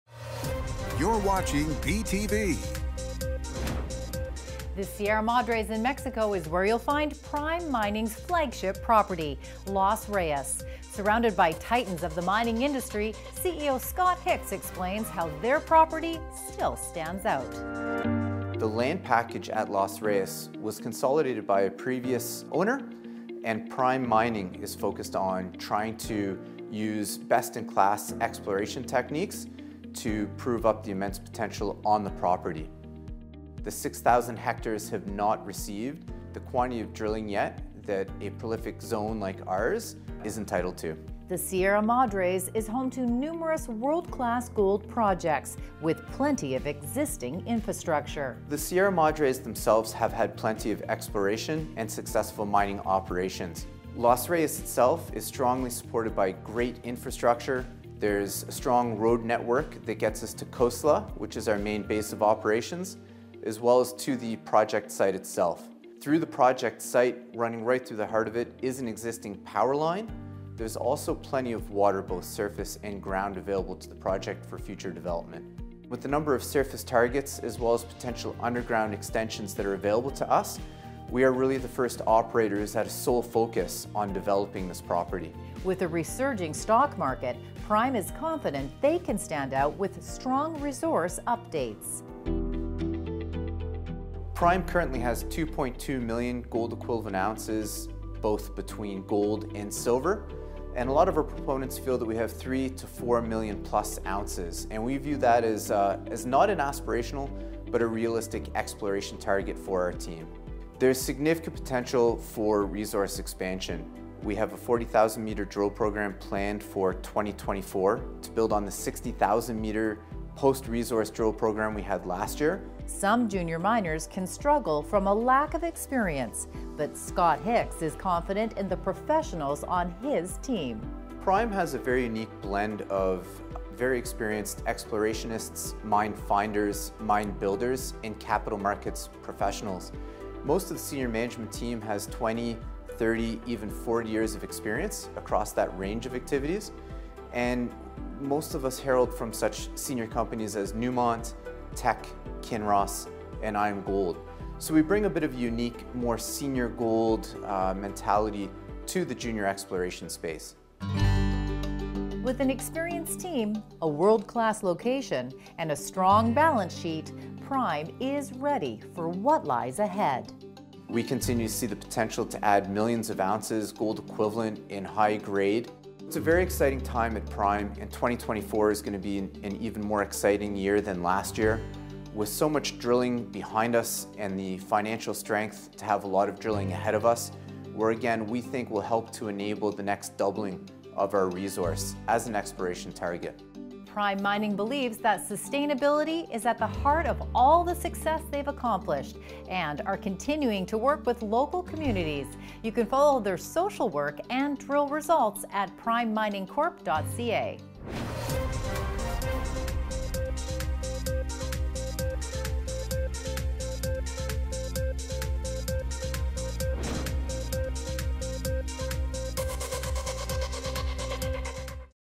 Exploring the Golden Opportunities with Prime Mining in the Heart of Mexico's Mining Sector
Investing in Mining Stocks

Prime Mining stands out in the competitive mining industry with its flagship property, Los Reyes, in the Sierra Madres, showcasing a blend of advanced exploration techniques, a strong commitment to sustainability, and a seasoned management team poised for significant growth.

Nestled within the Sierra Madres of Mexico, Prime Mining's flagship property, Los Reyes, emerges as a beacon of untapped potential amidst the mining titans. CEO Scott Hicks sheds light on the distinctive features that set Los Reyes apart, emphasizing the company's dedication to best-in-class exploration techniques to uncover the vast riches beneath its 6000 hectares. Despite being in a prolific zone surrounded by world-class gold projects and robust infrastructure, Los Reyes has yet to receive the drilling intensity it deserves, positioning Prime Mining for a groundbreaking journey ahead.

The strategic focus on developing Los Reyes is evident in the meticulous consolidation of the land package by a previous owner, now leveraged by Prime Mining to explore the multitude of surface and potential underground extensions. With a foundation of great infrastructure, including a strong road network, existing power lines, and ample water resources, Los Reyes is primed for exploration and future development.

Prime Mining is not just about exploring gold and silver; it's about doing it right with a sustainable mindset. The company's ethos, deeply ingrained in sustainability, extends beyond exploration to working closely with local communities, ensuring that every ounce of gold is mined responsibly. As we head into 2024, Prime Mining is buoyed by a re-surging stock market and robust financial health, ready to embark on a 40,000-meter drill program targeting the expansion of its resource estimate, demonstrating not just aspirational but achievable targets for the team’s skilled explorationists, mine finders, and capital markets professionals.

As Prime Mining stands on the brink of discovery, with expectations of significant resource expansion and a commitment to environmental stewardship, the journey of Los Reyes is one of exploration, sustainability, and community partnership. For investors and enthusiasts alike, Prime Mining represents a golden opportunity in the heart of Mexico's Sierra Madres, ready to unveil the full potential of Los Reyes.

‍Prime Mining Corp. (TSX: PRYM, OTCQX: PRMNF).